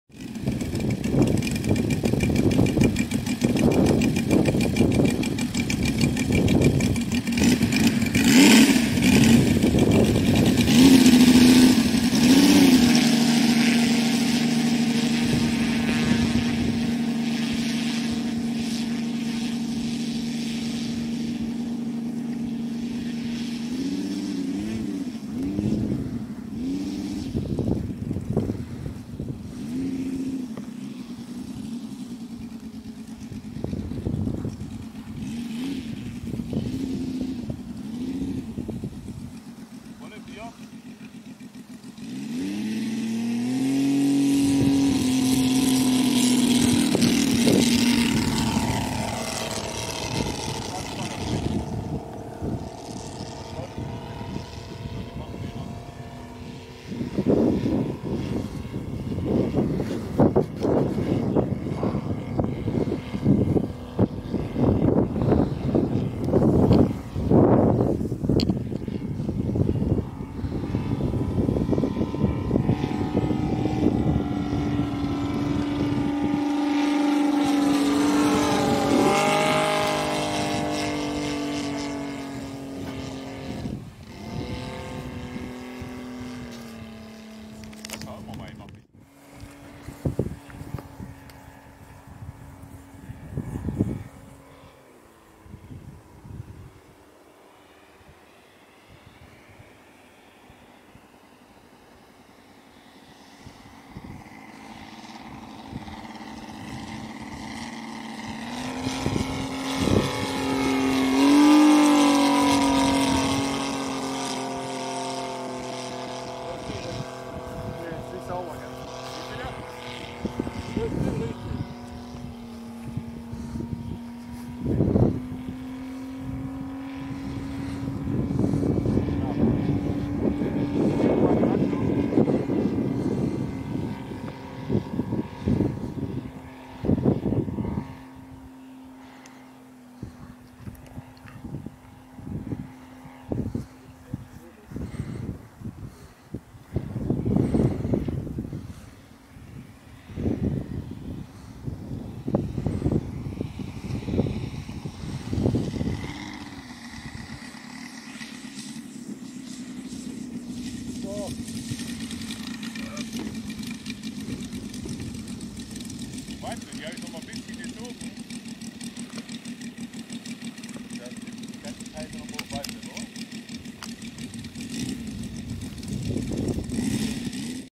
Frank's Zero beim MfV Sömmerda
Lauscht dem tollen Sound des Saito FG 84 3-Zylinder Motors!